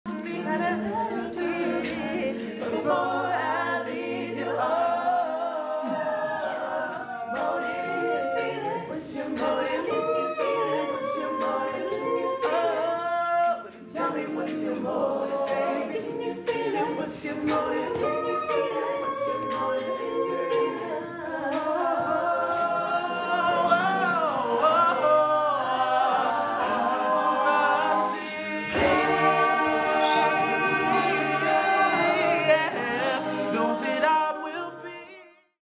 Class Notes heads to finals in international a cappella championship 🎶
For the first time, the Class Notes – Cornell’s oldest all-gender a cappella group – will compete in the International Championship of Collegiate A Cappella Finals, making it one of the top eight of 450 registered collegiate a cappella groups and giving the group a shot at the first-place title.